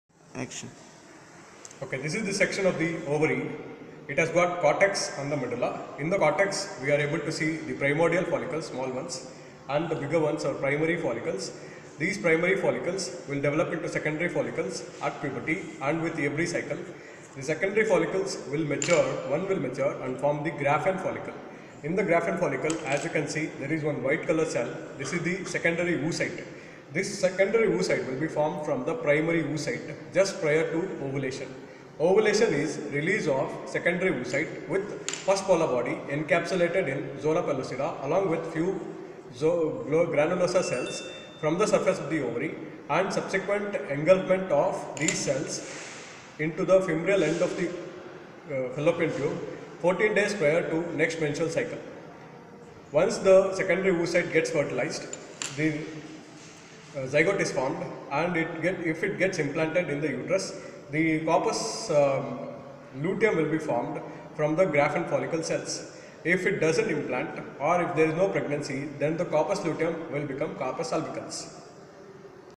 Ovary model showing follicles
Ovarian follicles

1. Primordial follicle: primary oocyte with single layer of flat follicular cells.

2. Primary follicle: Primary oocyte with single layers of cuboidal or columnar follicular cells; Zona pellucida present

3. Secondary follicle: Primary oocyte with two distinct surroundings – inner granulosa cells and outer theca cells

4. Tertiary follicle: Primary Occyte with surrounding corona radiata cells at one pole of follicle with antral fliud.

5. Graafian follicle: Mature tertiary follicle